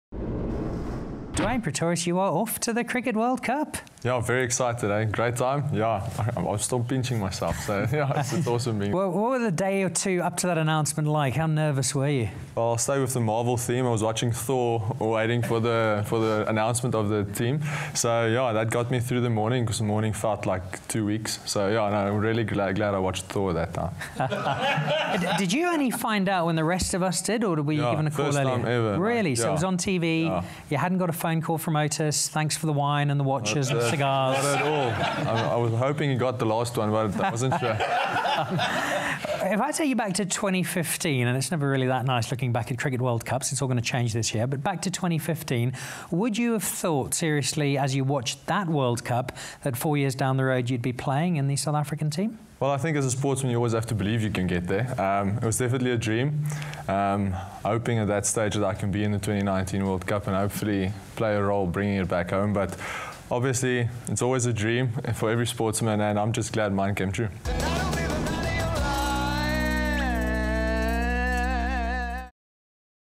Dwaine Pretorius's World Cup nerves
For most people, he was a guaranteed selection for the Cricket World Cup, but Proteas all-rounder Dwaine Pretorius was still a bundle of nerves before the squad announcement.